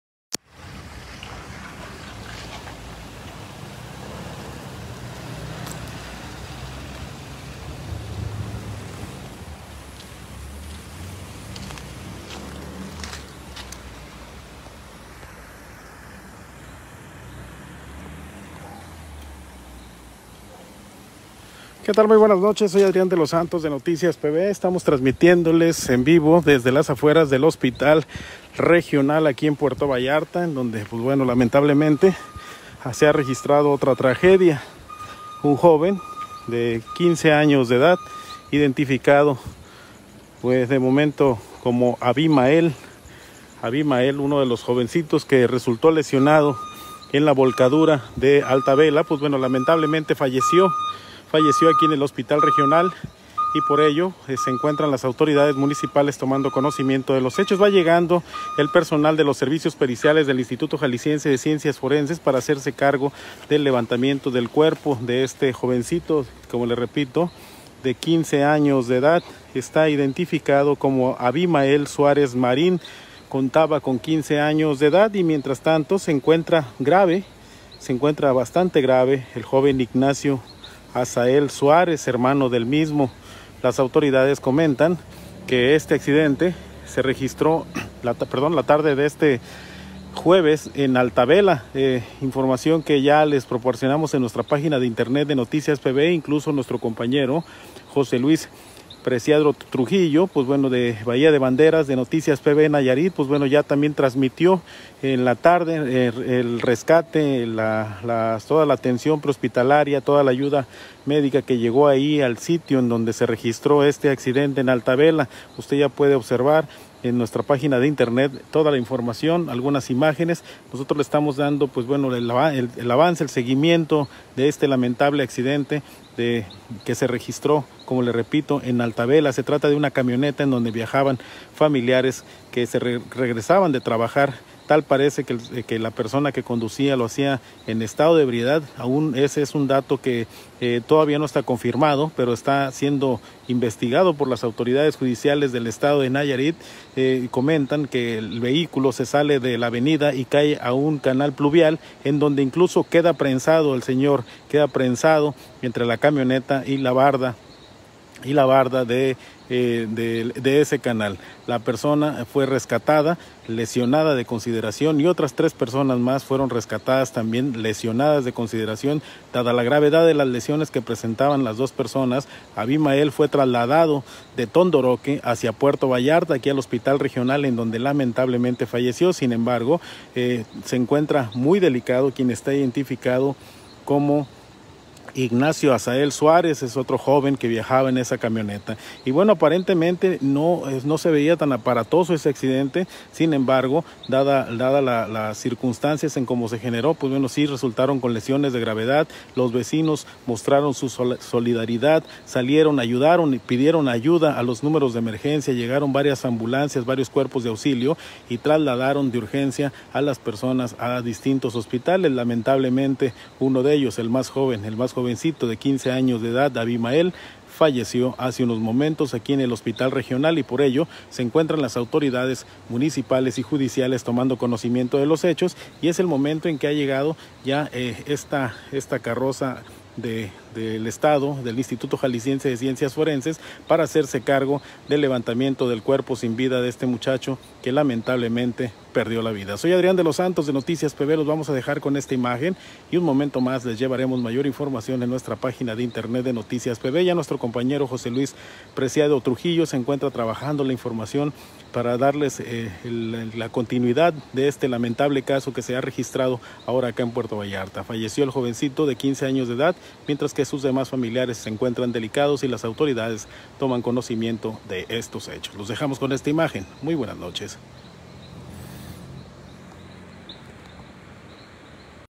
Lamentable fallecimiento de un adolescente
Momentos en que las autoridades municipales y judiciales en Puerto Vallarta, toman conocimiento acerca del lamentable fallecimiento de un adolescente que fue internado de urgencia en el Hospital Regional, tras haber sufrido lesiones al accidentarse la camioneta en la cual viajaba en el fraccionamiento Altavela, en Bahía de Banderas, otro joven se encuentra grave en el hospital de San Pancho. (Adrián De los Santos/NOTICIASPV.COM)